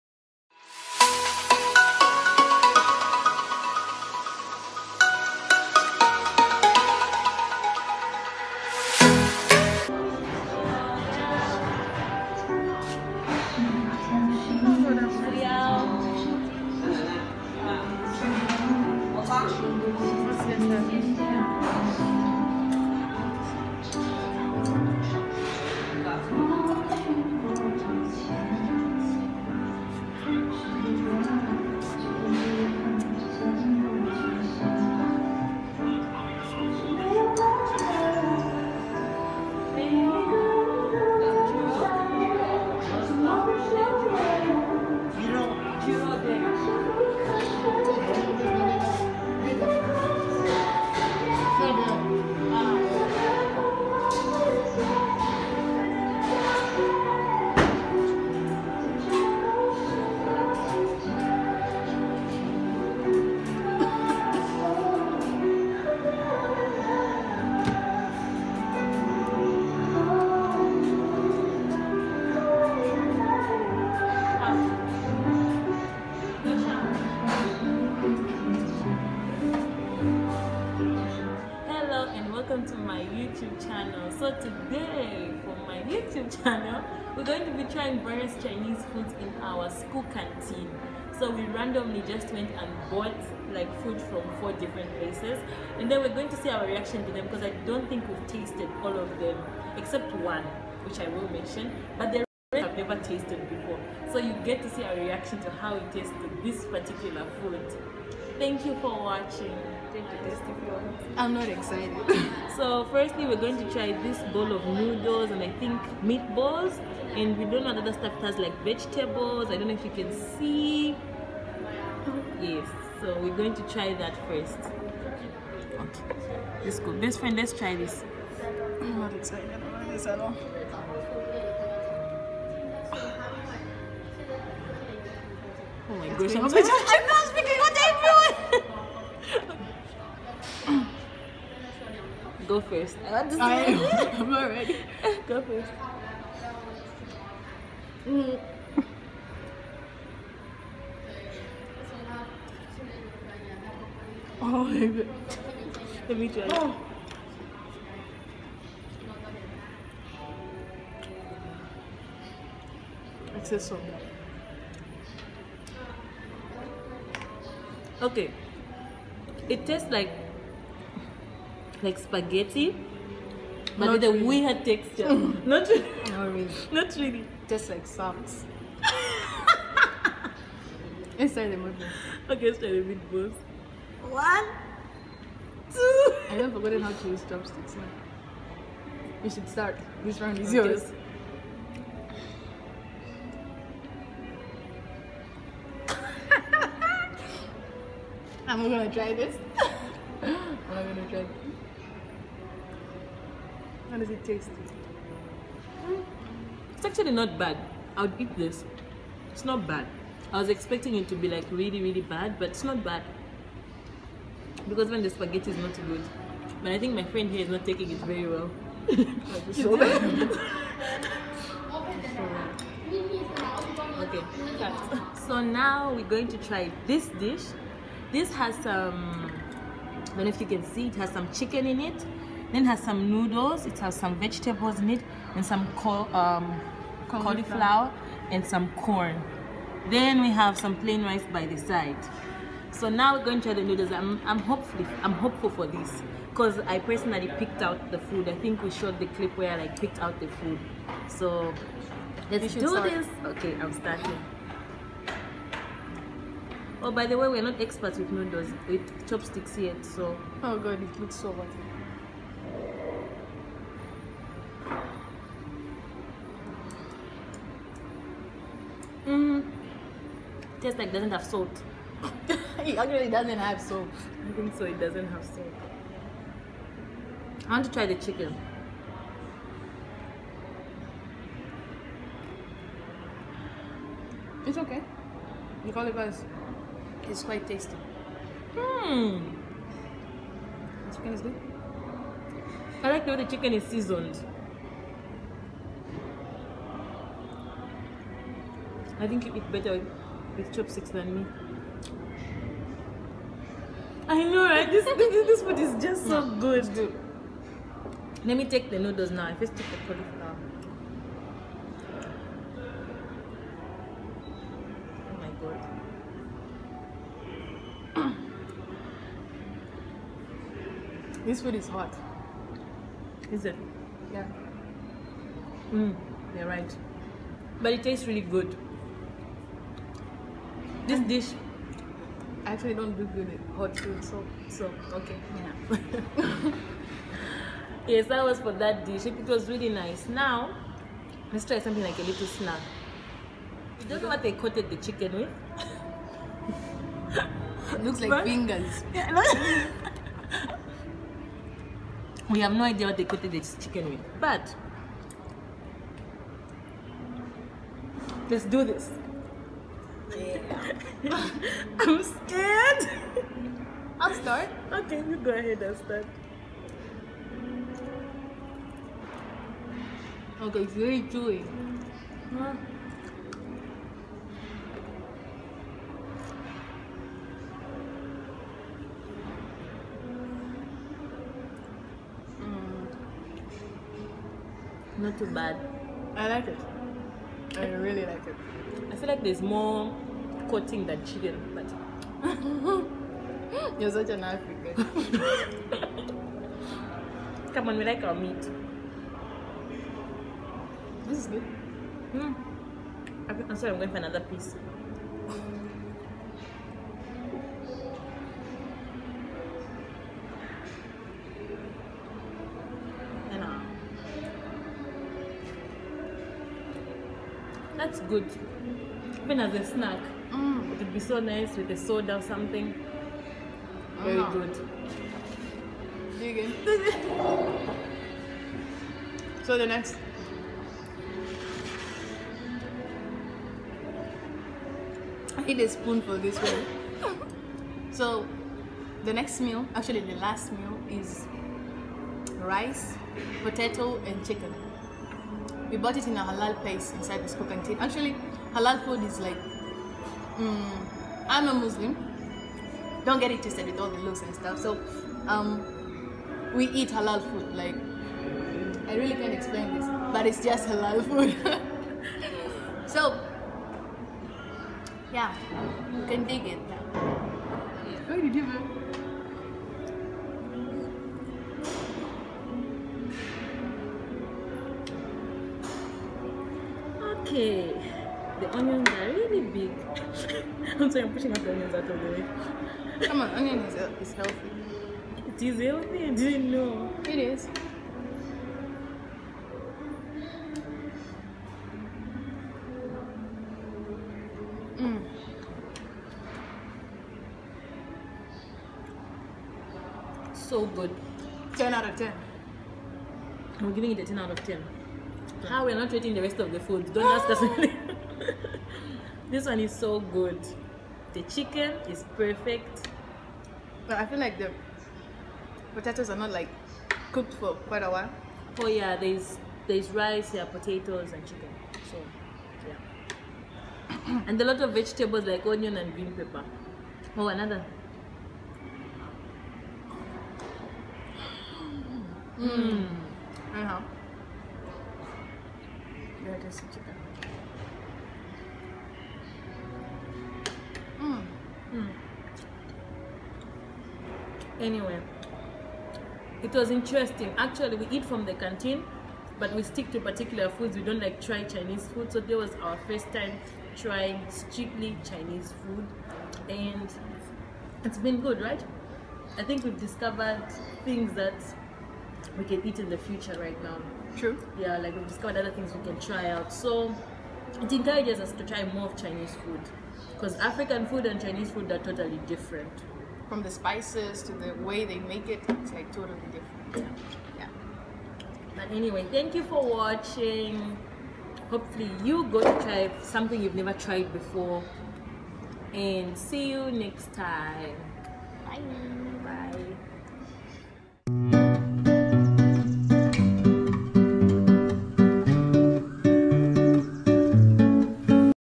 Trying the Chinese in school cafeteria | Zhejiang Chinese Medical University
My friend and I went to our school canteen/cafeteria and tried some Chinese food.